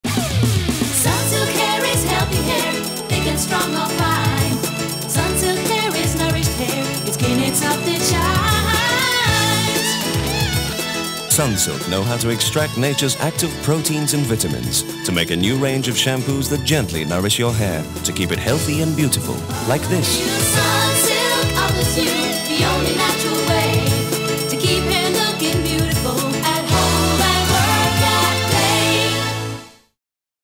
Sunsilk "Healthy" (English ver., v.1) - Arab GCC, 1998
Note: This is a version for English-language TV stations in the Arab World.
إعلان صانسيلك